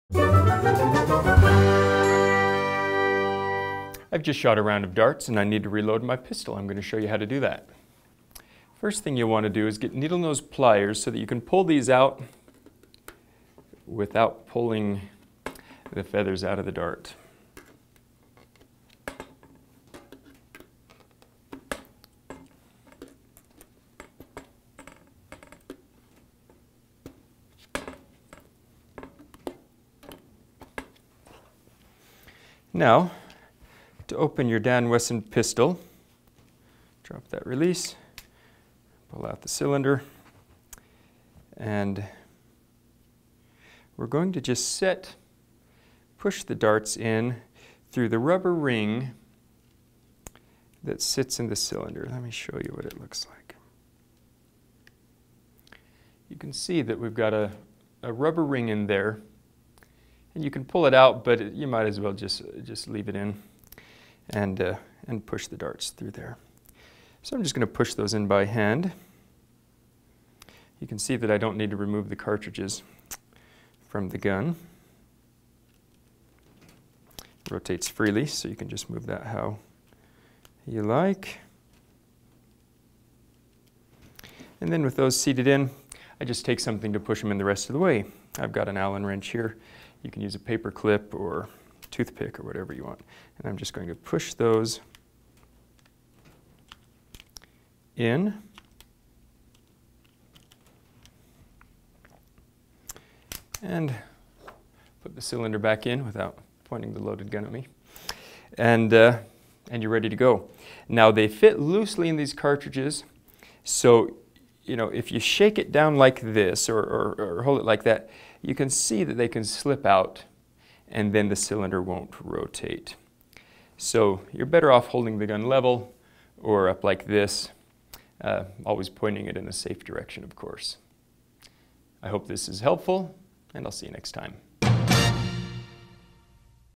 Airgun Darts: Loading the Darts
In this short video Travis shows you how to load airgun darts into CO2 revolver cartridges.